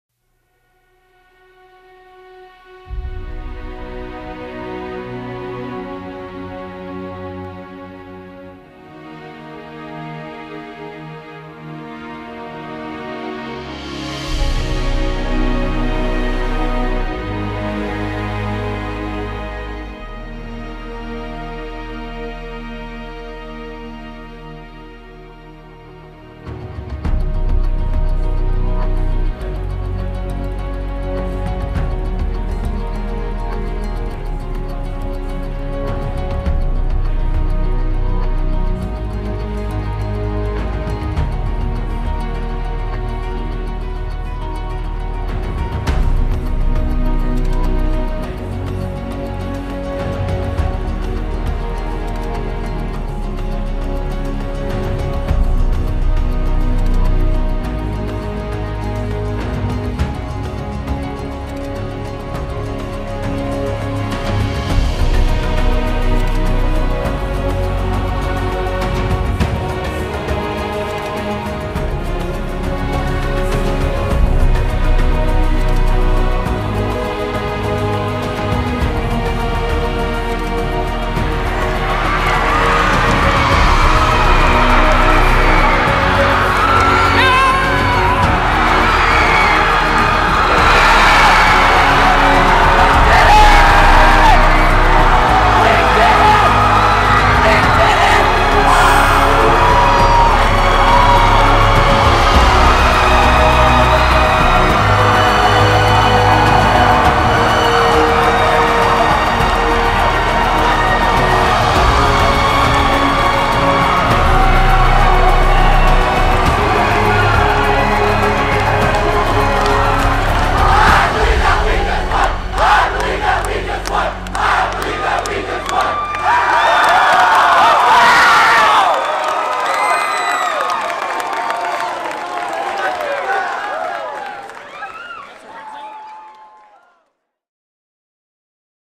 Shakopee Wrestling 2018 2AAA Section Champions (Highlights)
Shakopee Wrestling is heading to State for the first time since 2004!  After defeating perennial powerhouse (the Apple Valley Eagles) by a score of 34-30, the Sabers achieved the unthinkable, ending one of the most remarkable streaks in high school athletics.  The Minnesota State Wrestling tournament gets underway on March 1, 2018 at the Excel Energy Center.

Main Camera Set-up:
-Sony Camera
-Best Zoom Lens
-Best Wide-Angle Lens
-DJI Mavic (Drone)

GoPro Set-up: Ultimate Follow-Cam
-GoPro Hero 6
-Phone Mount